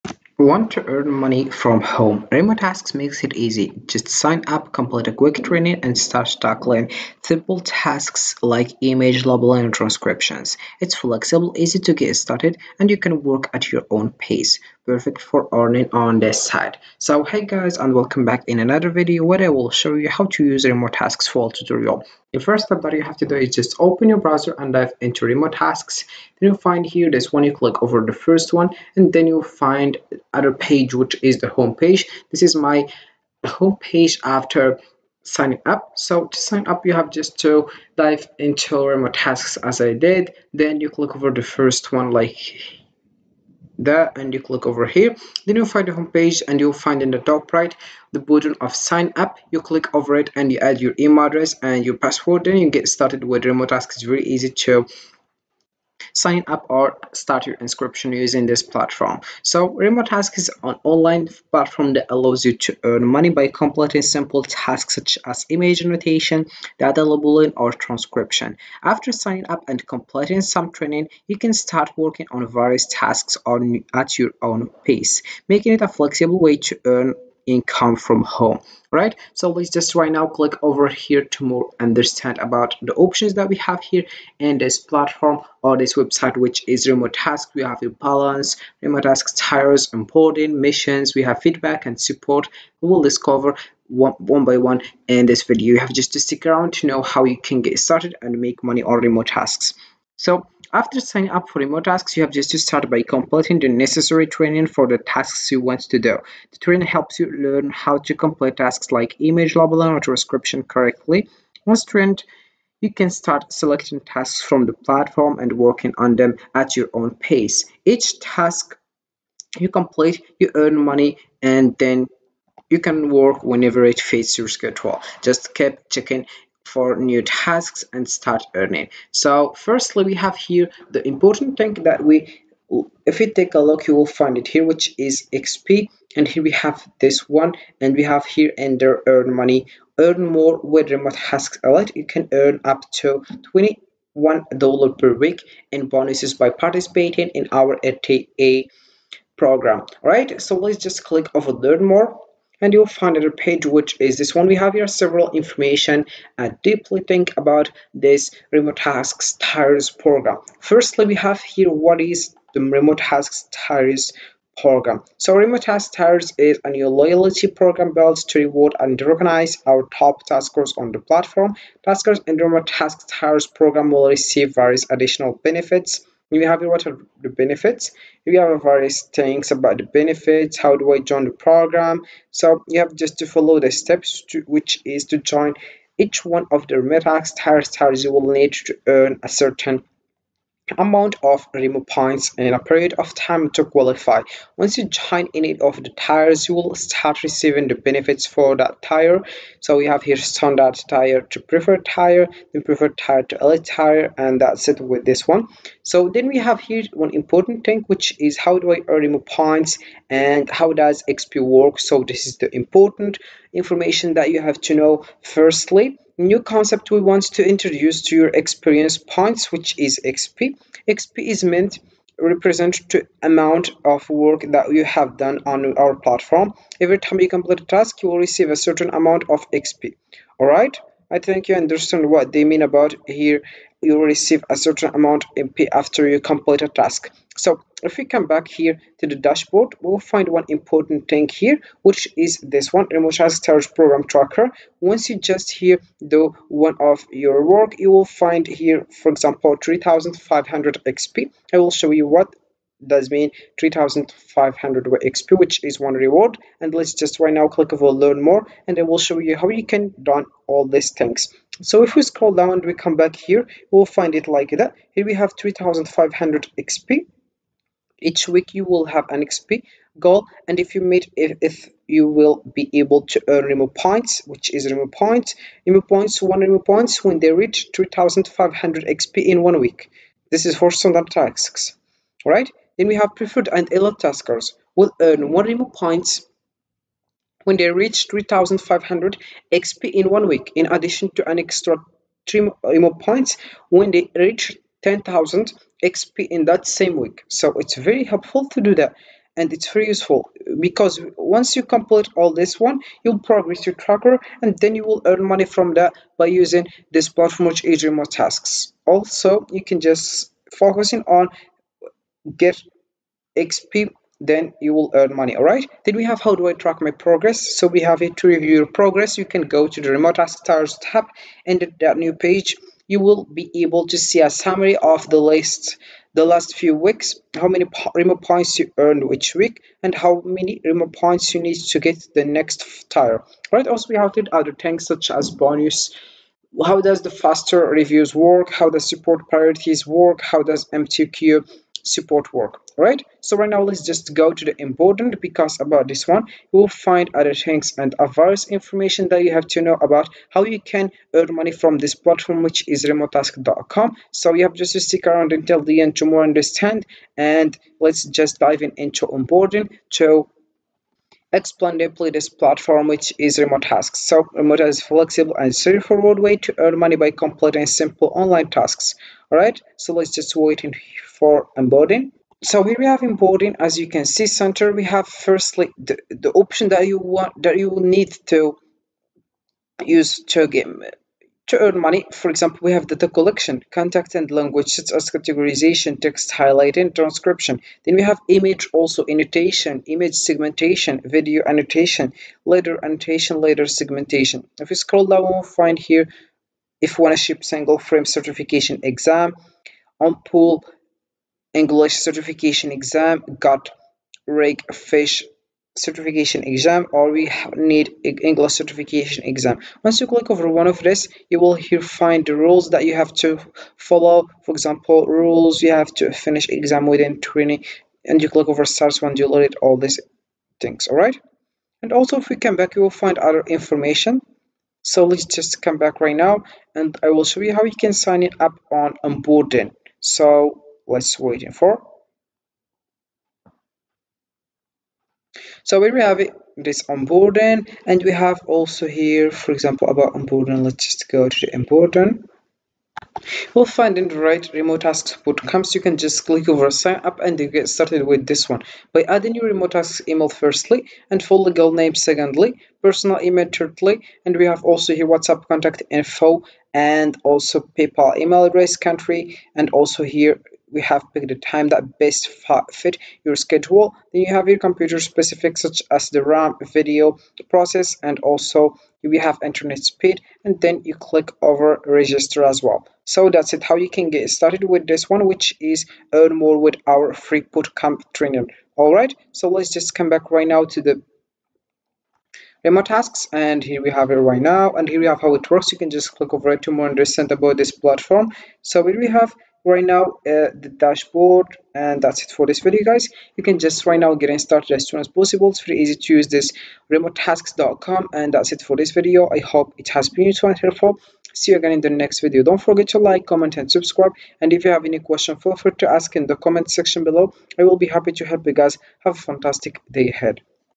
Remotasks Tips and Tricks for Beginners (2025 Edition)
in this video we discuss : Get started with Remotasks, a platform that lets you earn money by completing microtasks online. This beginner-friendly tutorial will guide you through account setup, task selection, and tips to maximize earnings. Learn how to work on Remotasks efficiently and boost your productivity, perfect for beginners in 2025!